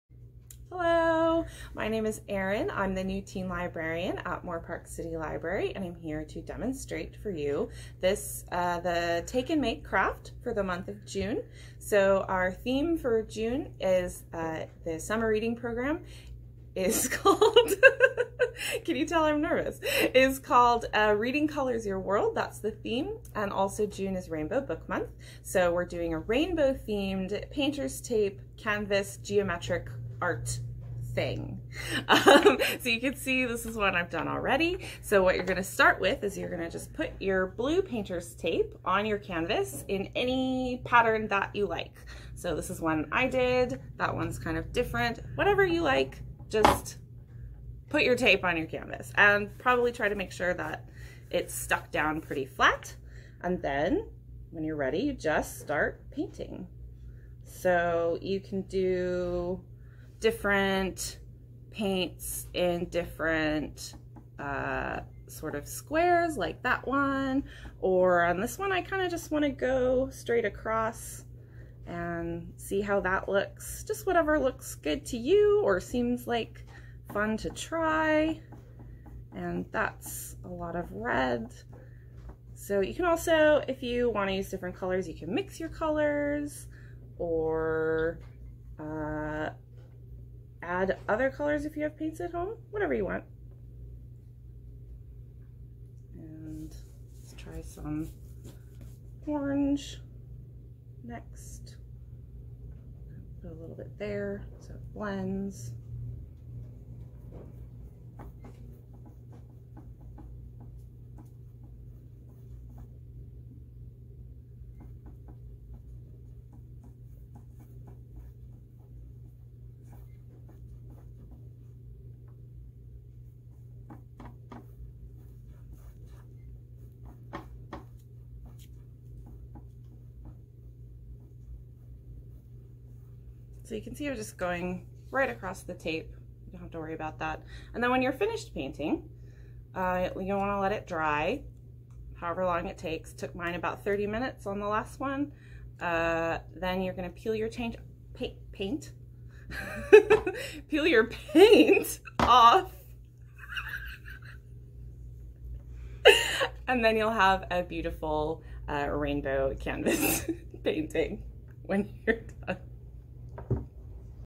Painter's Tape Canvas Art
Pick up your take & make craft kit from the Library to make a geometric painting using painter's tape on your canvas.